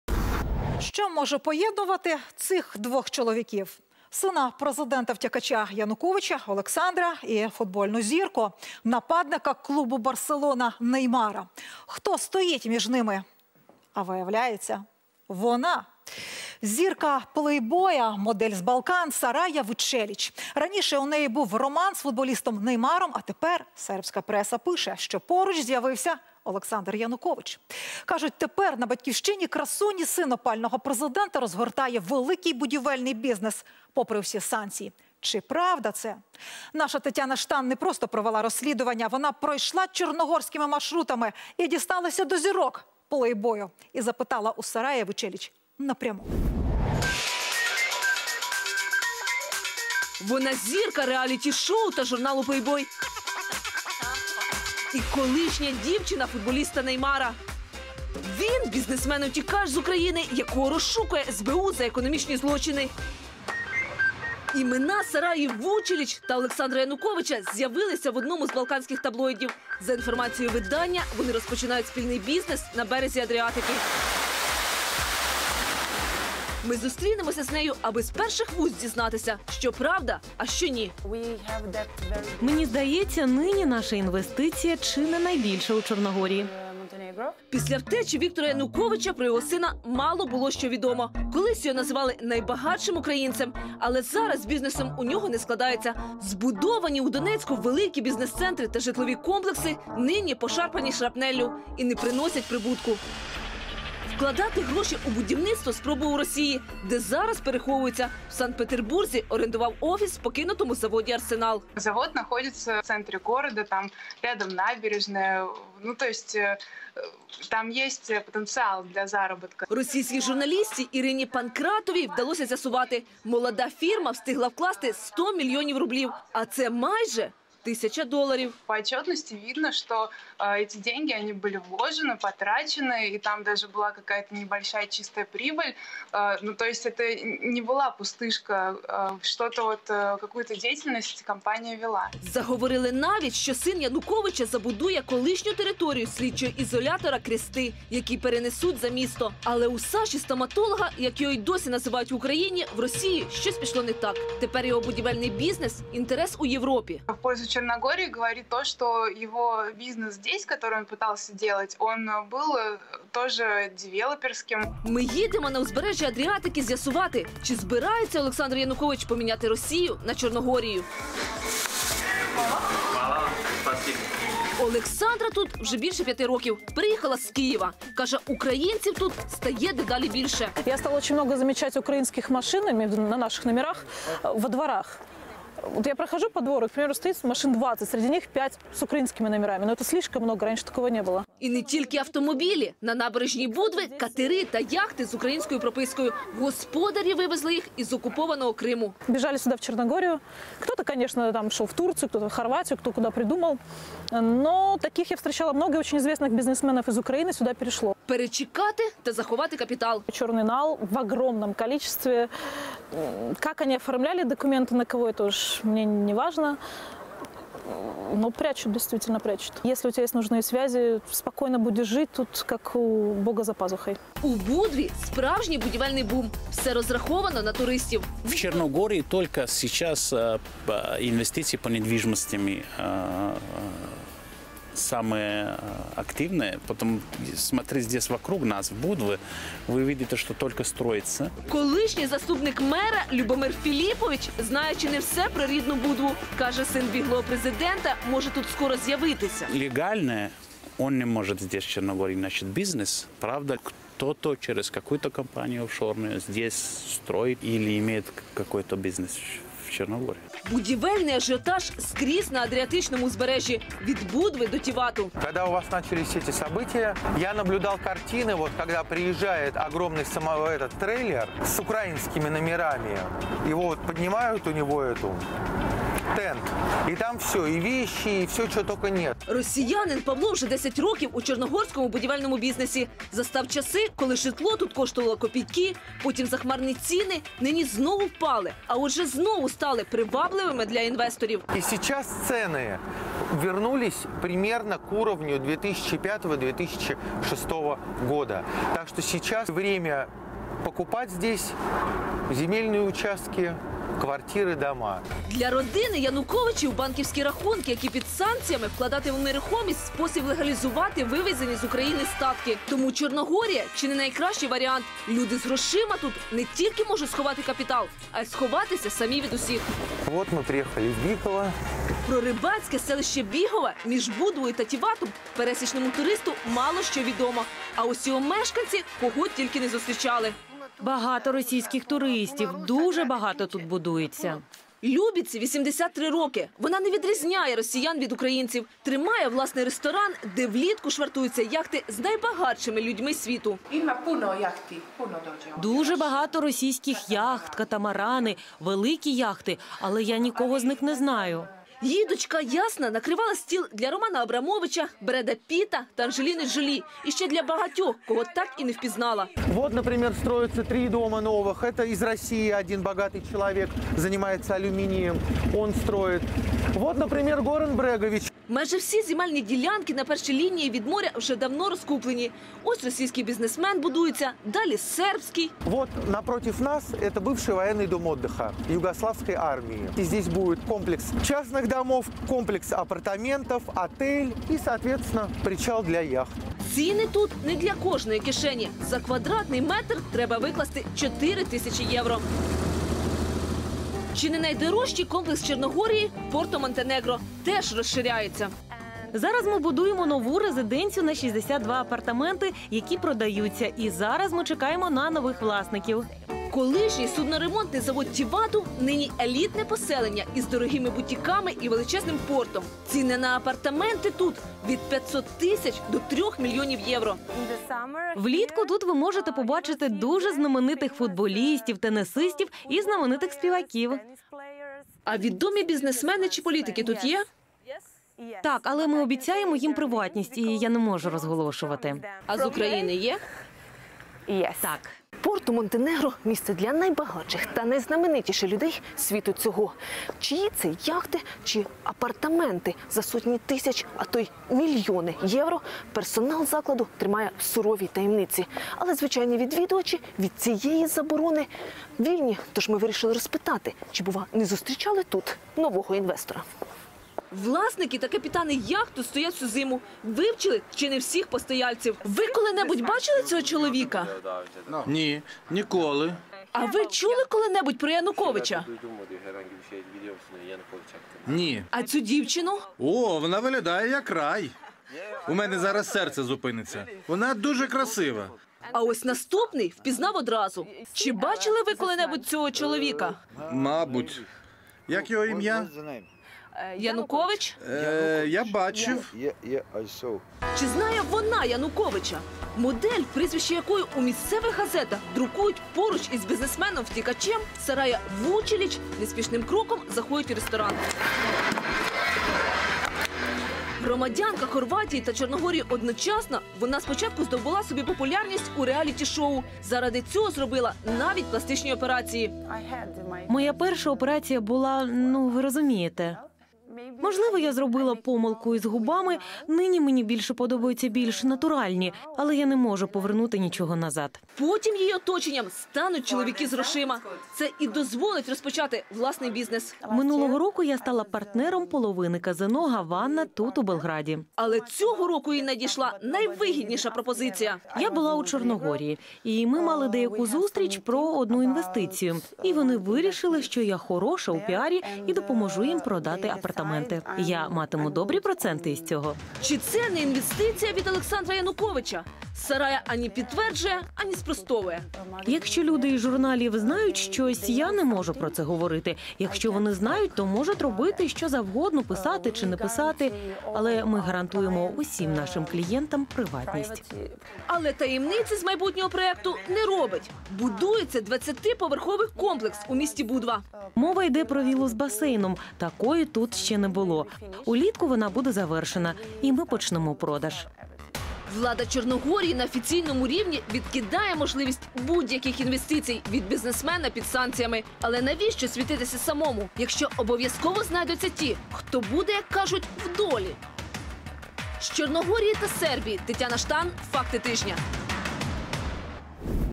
Модель Плейбоя из Сербии и Александр Янукович. Что их связывает?
Что может объединять этих двух мужчин? Сына беглого президента  Александра Януковича и футбольную звезду. Нападающего клуба "Барселона" Неймара? Кто стоит между ними? Наша Татьяна Штан провела расследование.